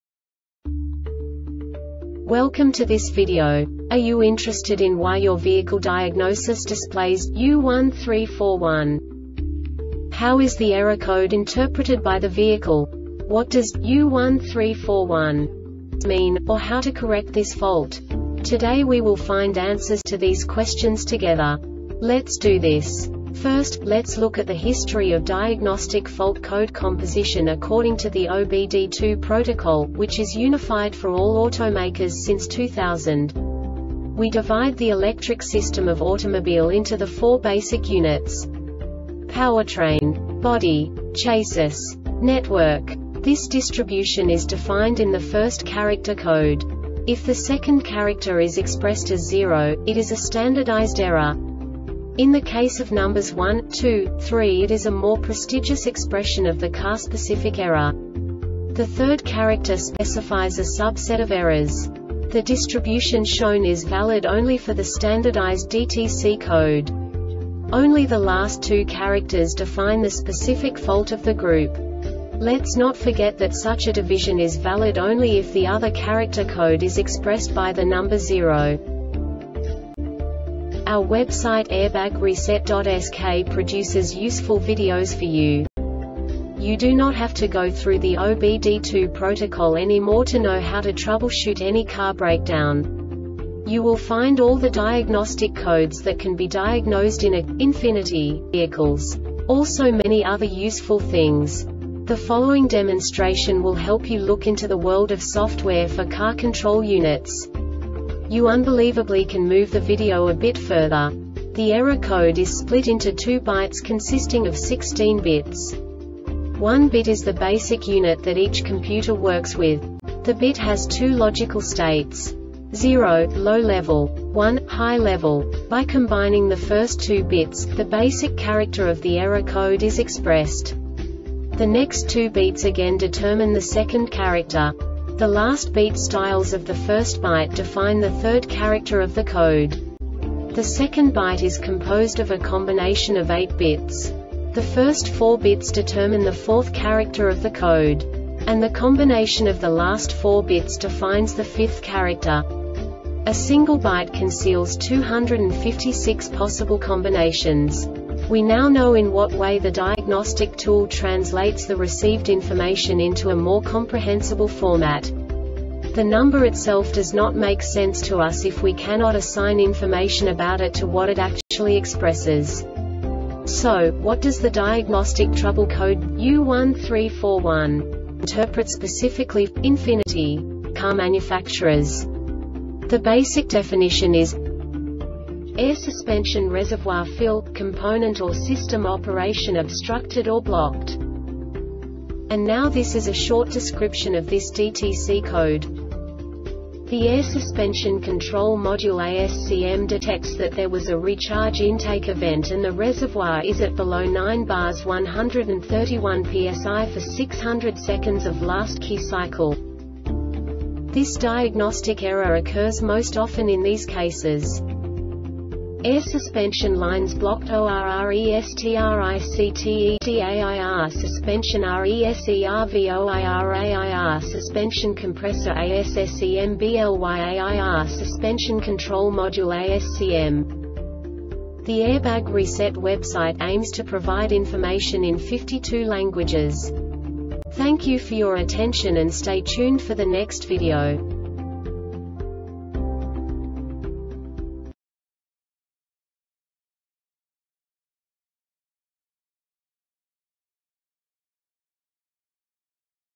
DTC Ford U1341 Short Explanation
Contents:
0:21 Basic DTC analysis according to OBD2 protocol standard.
1:48 Insight into programming
2:58 A diagnostically understandable description of the Diagnostic Trouble Code
3:05 Basic error definition

Make:
Ford
Code:
U1341
Definition:
Air Suspension Reservoir Fill - Component Or System Operation Obstructed Or Blocked
Description:
The Air Suspension Control Module (ASCM) detects that there was a recharge intake event and the reservoir is at below 9 bar (131 psi) for 600 sec of last key cycle.
Cause:
AIR SUSPENSION LINES BLOCKED OR RESTRICTEDAIR SUSPENSION RESERVOIRAIR SUSPENSION COMPRESSOR ASSEMBLYAIR SUSPENSION CONTROL MODULE (ASCM)